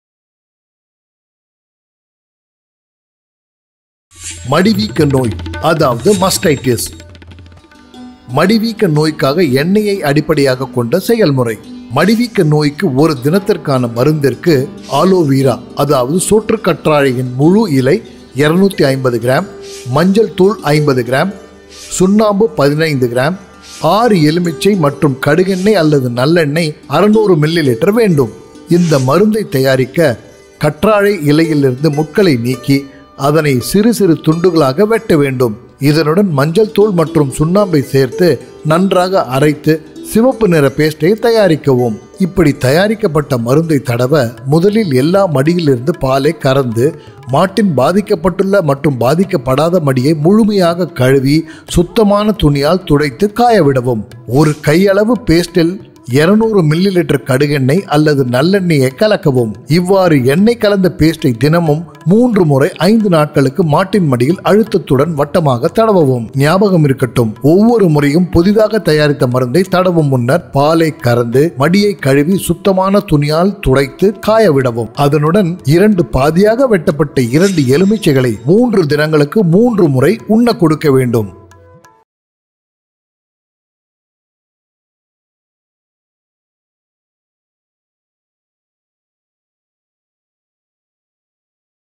Oil-based ethno-veterinary formulation for Mastitis (Tamil)
Mastitis, or inflammation of the udder, results in severe losses to the farmer due to drasticreduction or total loss in milk yeild. The video takes through an effective oil-based ethno-veterinary formulation for field implementation.

The common plants, spices and other materials mentioned here are generally regarded as safe and these are only suggestive. Nearby veterinarian may be consulted for proper disease diagnosis and management.